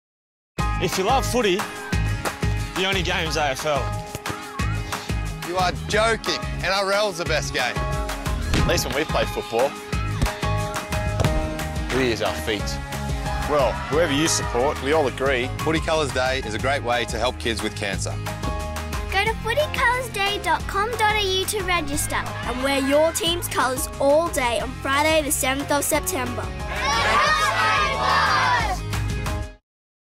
Footy Colours Day 2012 - TVC
Wear your favourite football team's colours to work or school and help support young people fighting cancer.